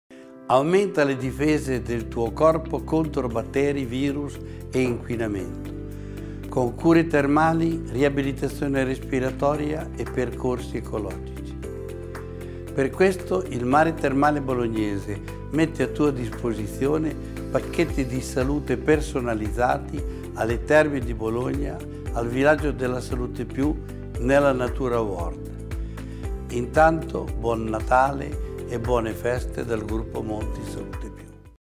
Salute e benessere per tutti alle Terme di Bologna
Il Mare Termale Bolognese mette a tua disposizione  pacchetti di salute personalizzati  alle terme di Bologna e al Villaggio della Salute Più – Natura World
con 55 percorsi ecologici … e intanto Buon Natale dal Gruppo Monti Salute Più.